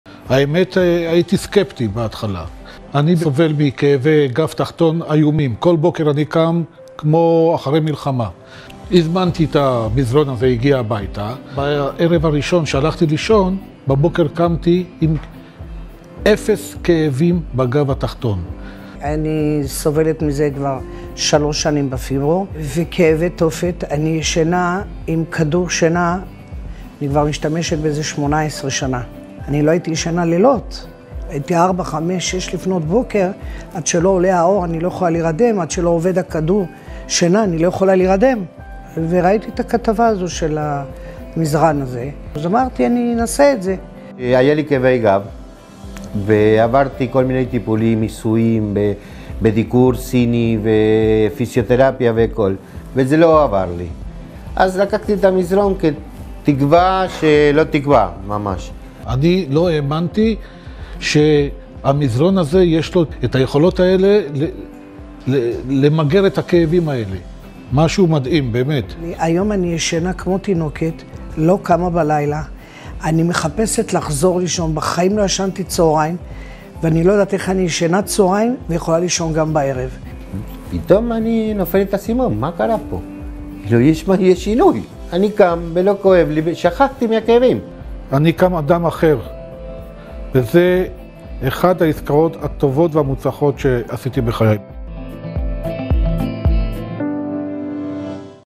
מגנייט - עדויות 4 לקוחות מנהריה שסבלו מפיברומיאלגיה וכאבי גב ובזכות מזרן מגנייט חייהם השתפרו פלאים
הקשיבו לעדויות של ארבעה לקוחות שונים מנהריה שסבלו מפיברומיאלגיה, כאבי גב ובעיות שינה ובזכות השכיבה והשינה על המזרן המגנטי של מגנייט חייהם השתנו מקצה לקצה.
גם לכם כדאי לנסות את מגנייט - קחו את המזרן ל-30 ימי ניסיון ותראו ותרגישו איך הוא משפיע עליכם.